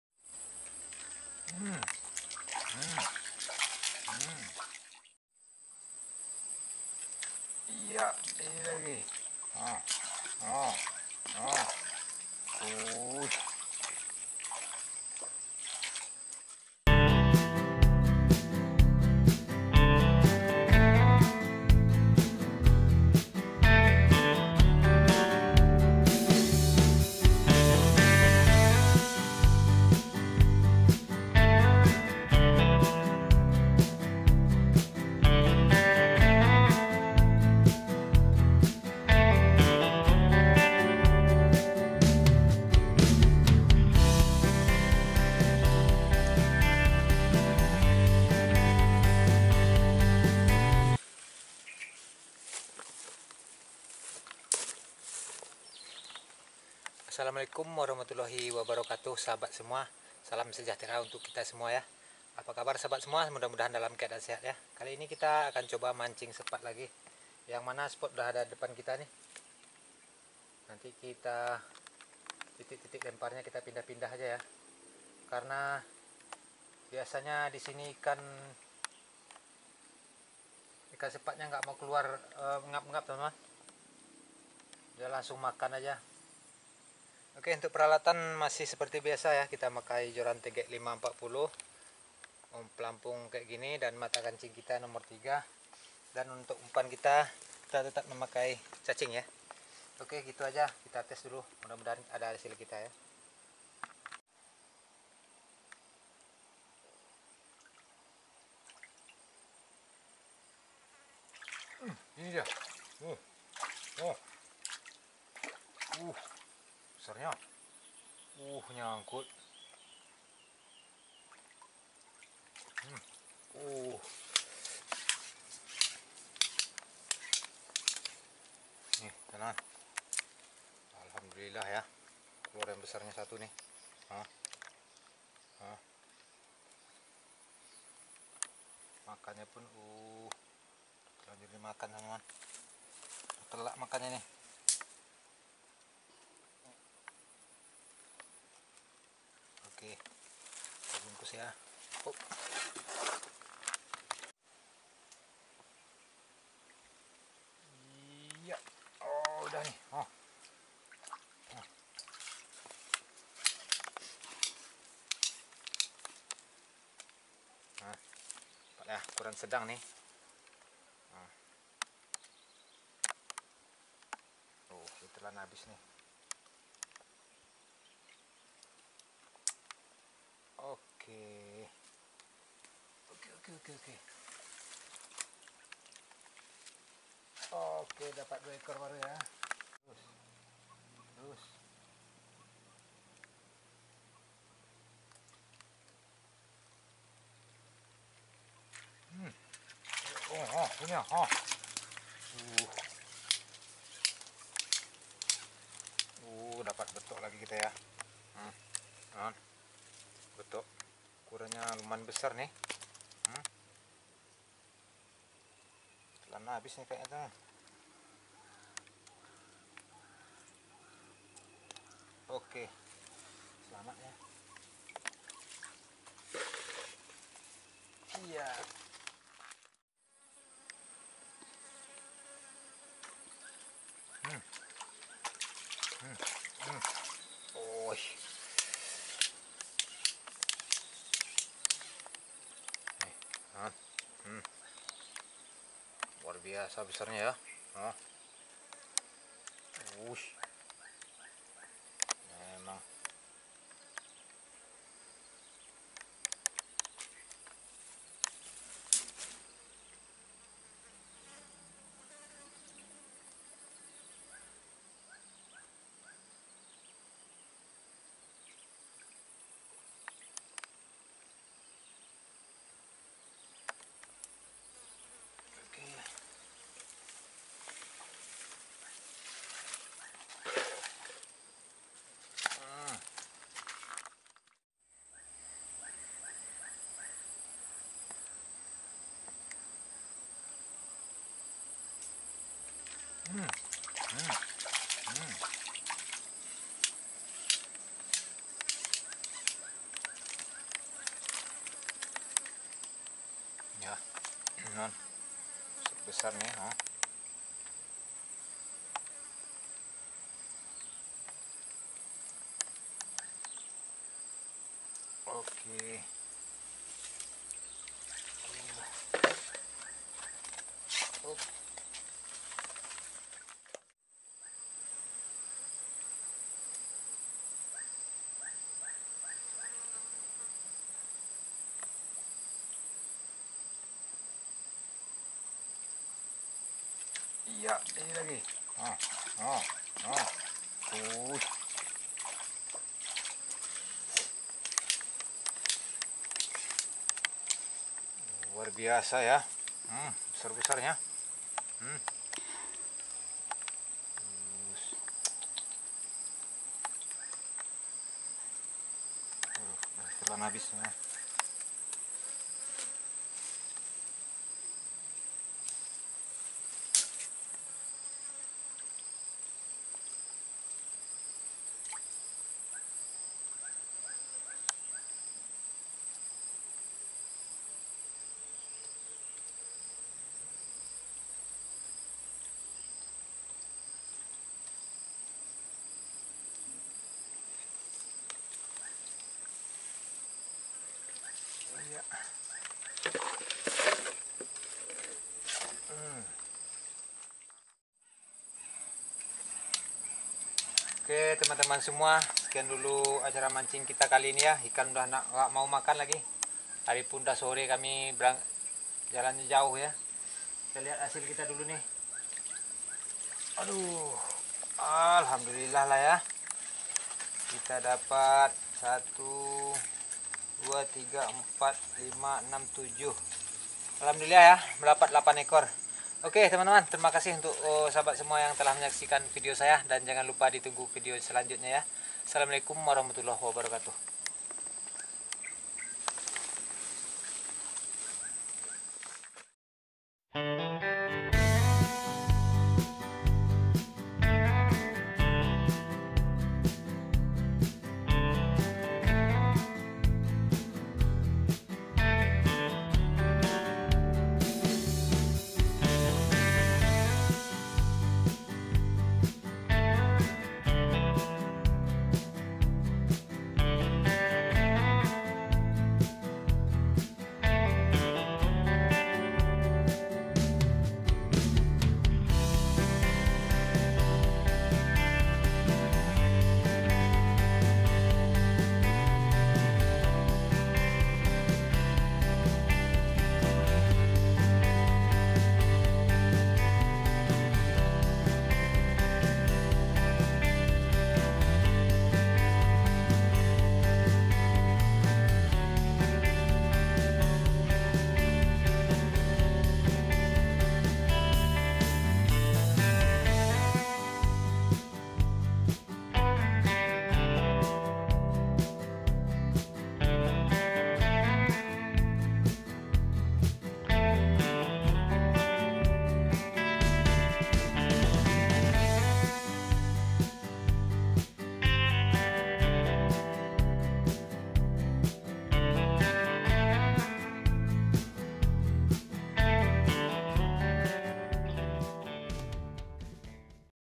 MELIMPAH,,!!entah berpa banyak sepat siam di sini#TR-040
spot mancing sepat Siam di pinggiran danau mati
halo sobat semua kita mancing sepat Siam di pinggiran danau mati

peralatan
joran tegek
mata pancing no 03
umpan cacing air